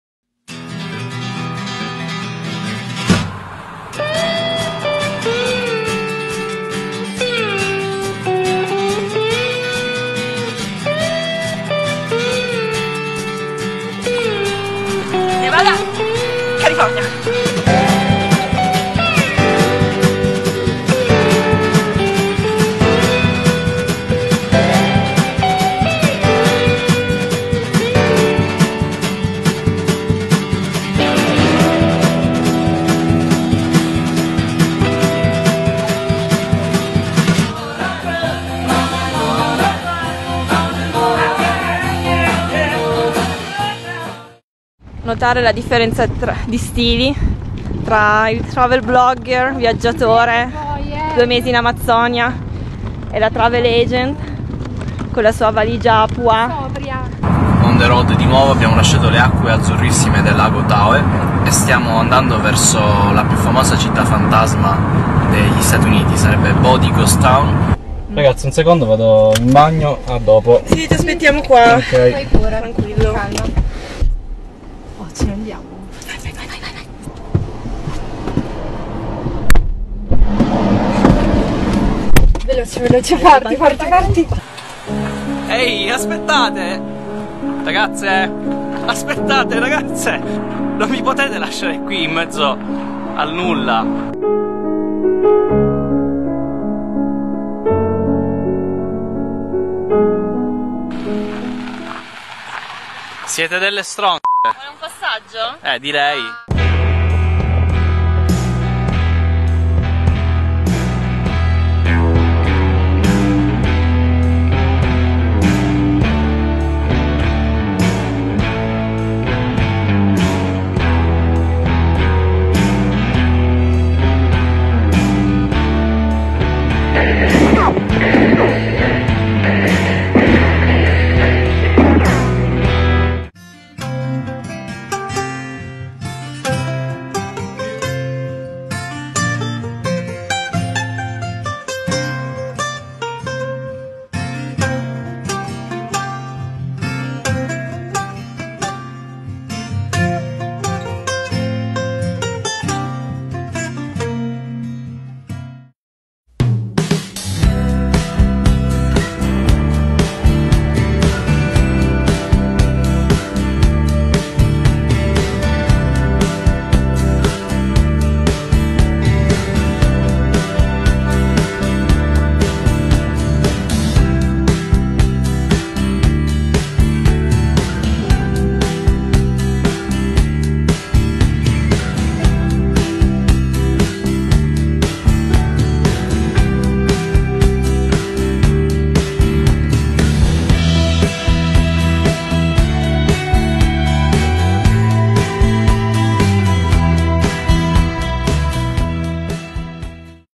CaliforniaDream - da South Lake a Mammoth Lakes (Episodio 4)
CaliforniaDream - Episodio 4: da South Lake a Mammoth Lakes.

Una quarta tappa a tutta natura per i viaggiatori di CaliforniaDream. Fabio, Monica ed Erica ci portano a South Lake e Mammoth Lakes, tutto rigorosamente "on the road".